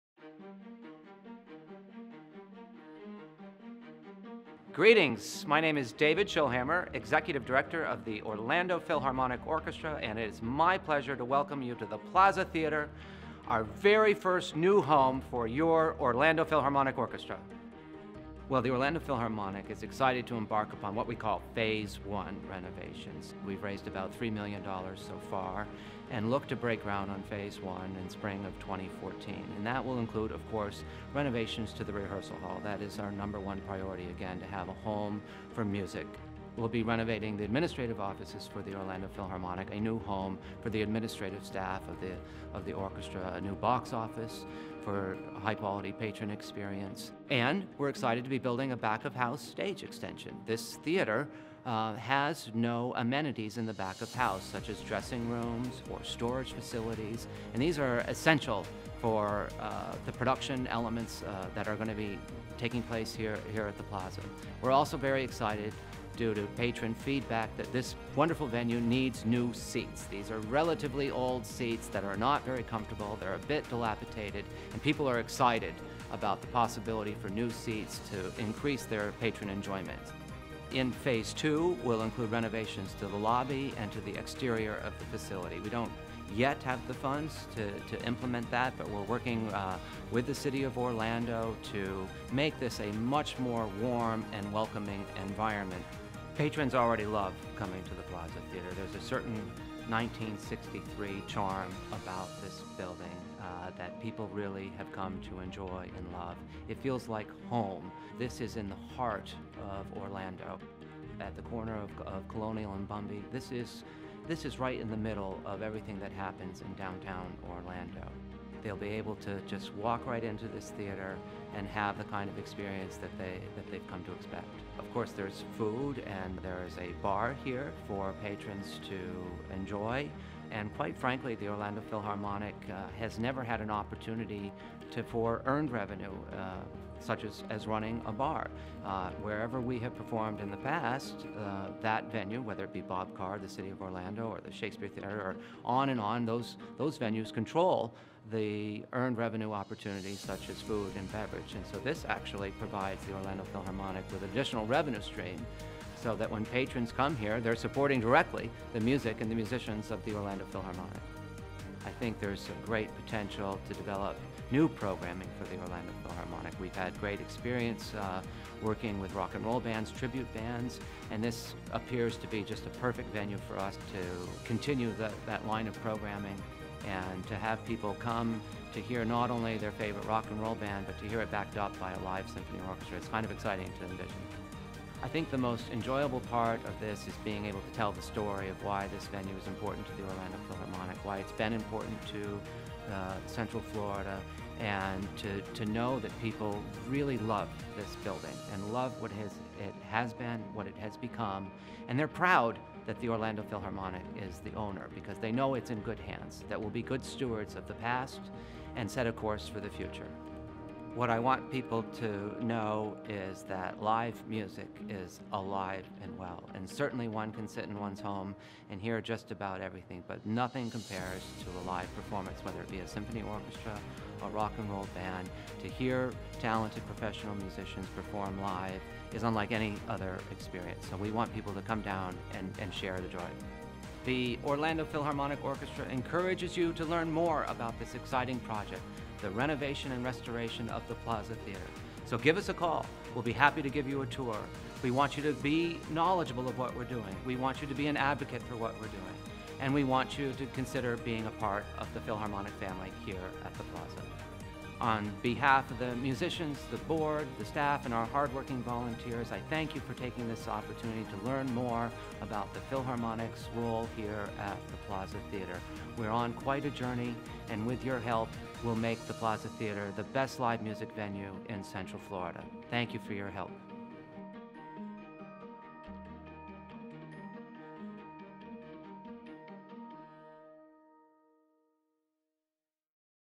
Executive Director David Schillhammer on the Plaza Live Project
Orlando Philharmonic Executive Director David Schillhammer introduces the orchestra's plans for the Plaza Live in downtown Orlando, a building that will be renovated to become the Philharmonic's new home, while continuing to operate as one of Central Florida's favorite live music venues.